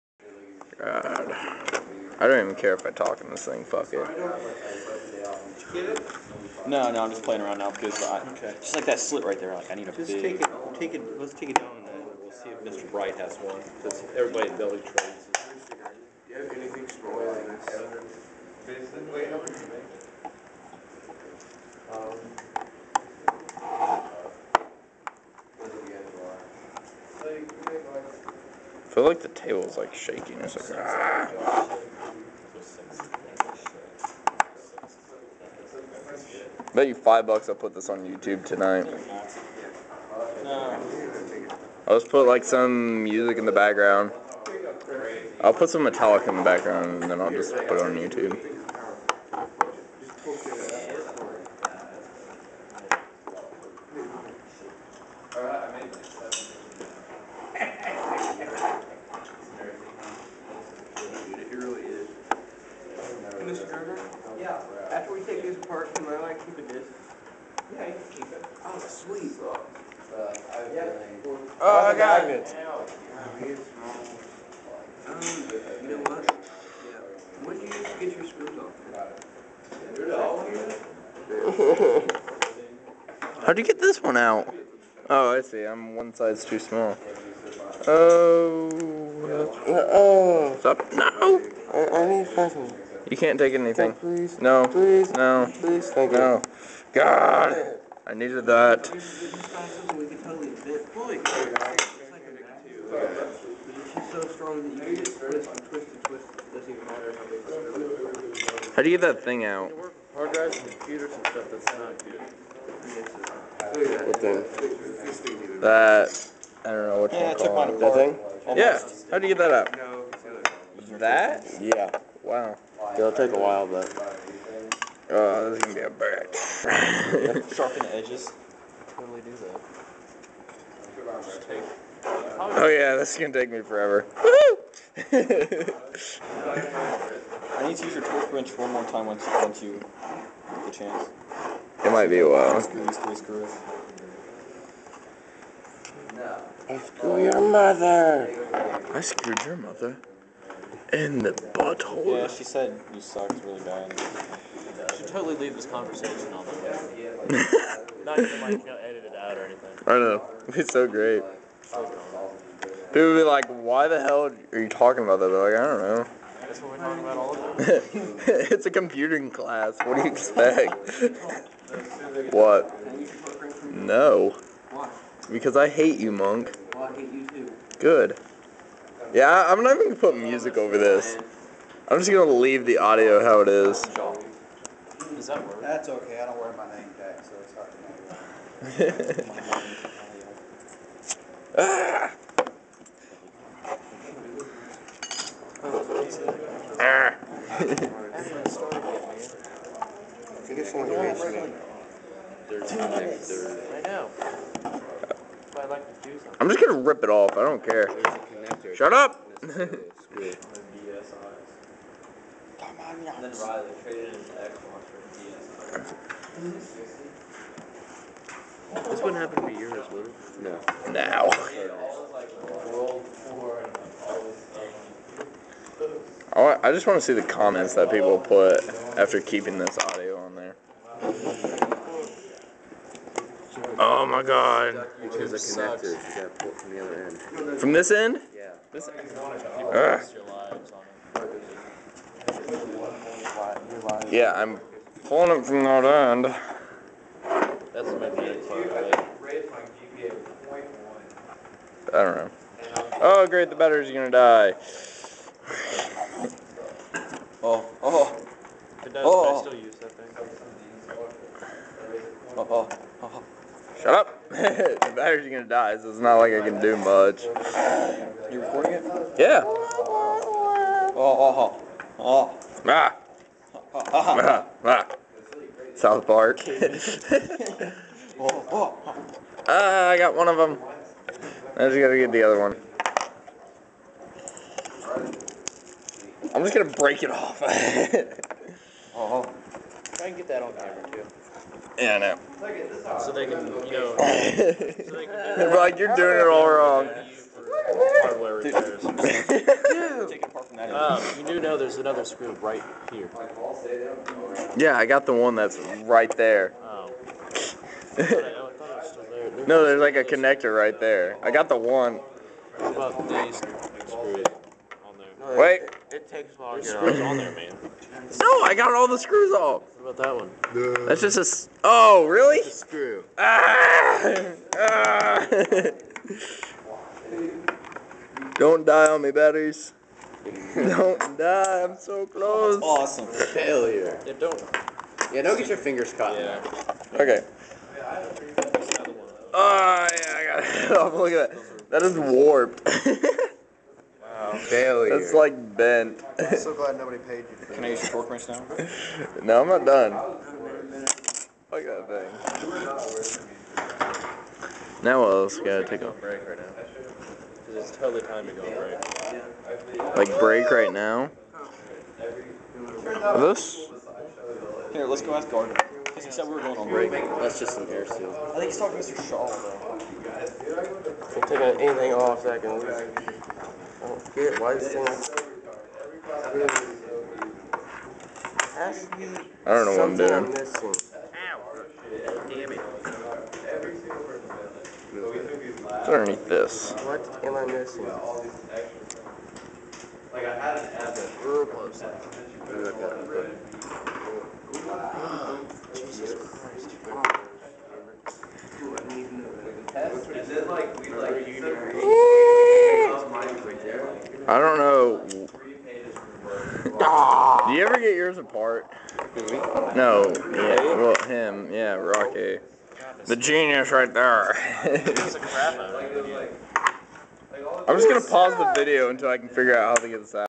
Taking apart my first Hard Drive Part 1
taking apart a hdd at vocational school